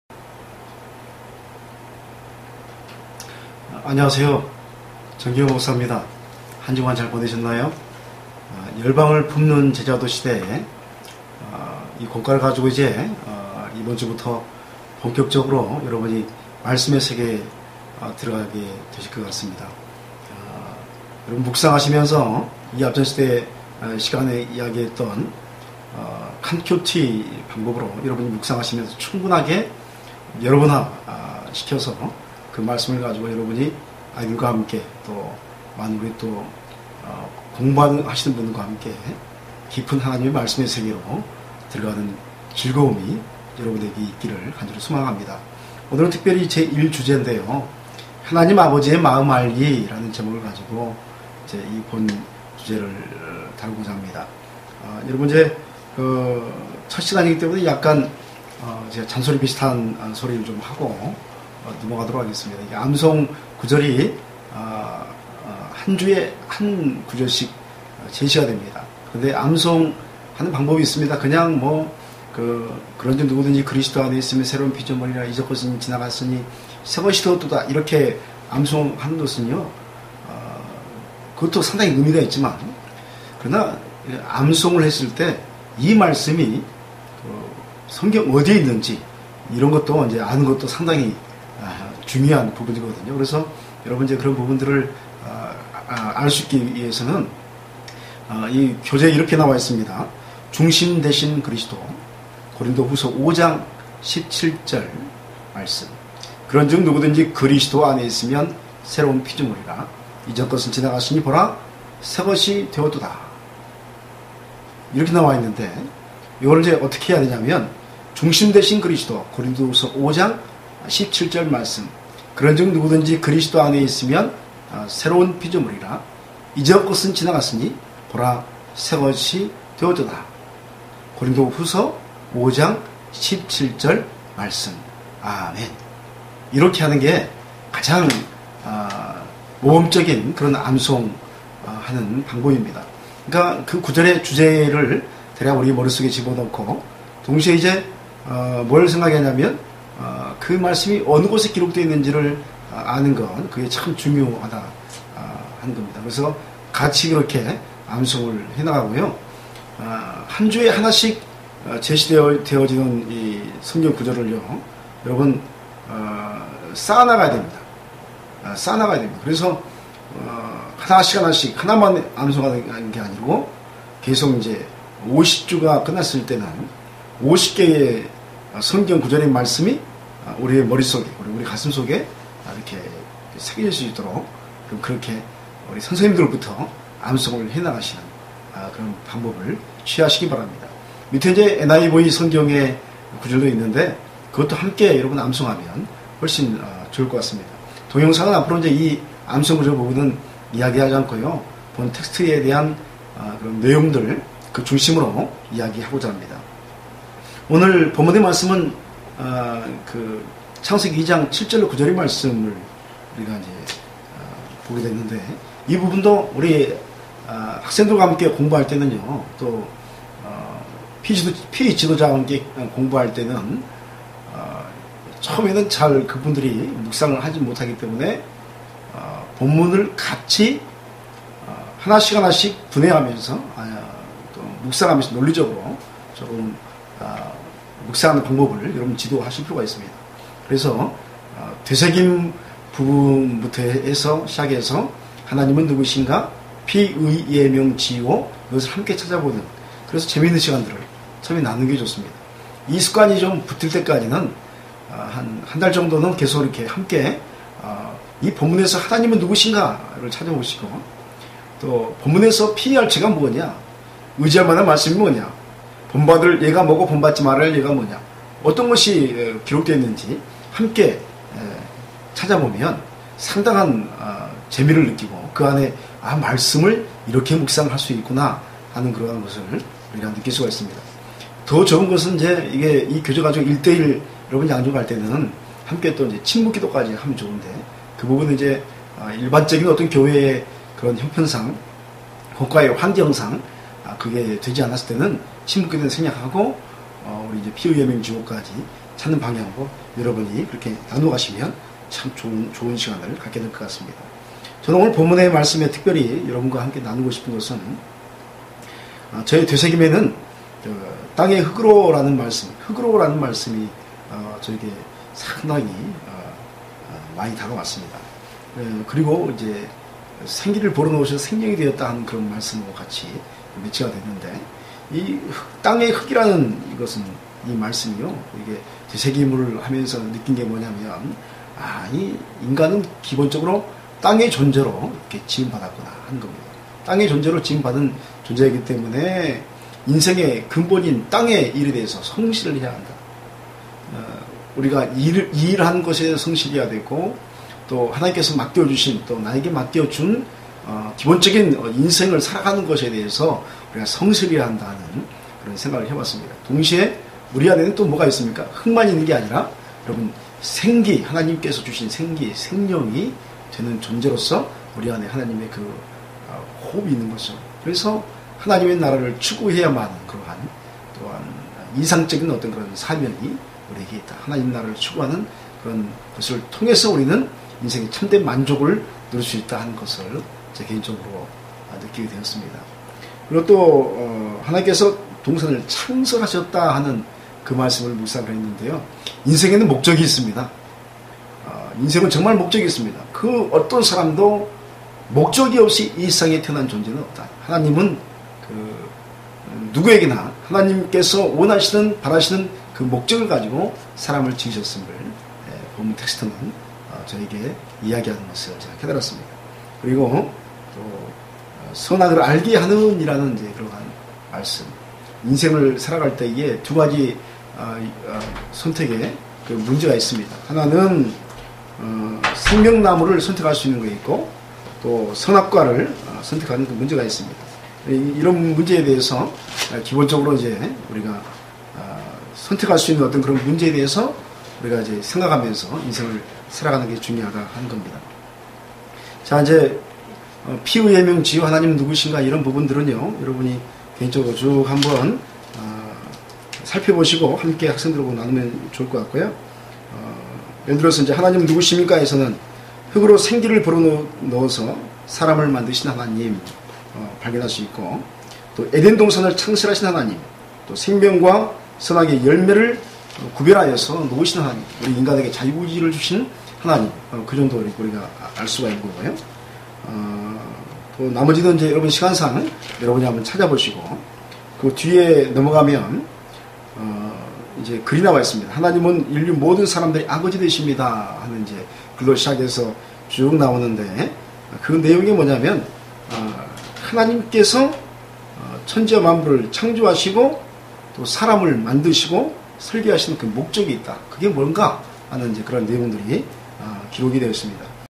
열방을품는제자도시대02 / 제 1 주제 하나님 아버지의 마음 알기 / 지심 정경호 목사
샤마임 영성 공동체 대표 지심 정경호 목사의

열방을 품는 제자도 시대 50주 공과 교재의 동영상 강의입니다.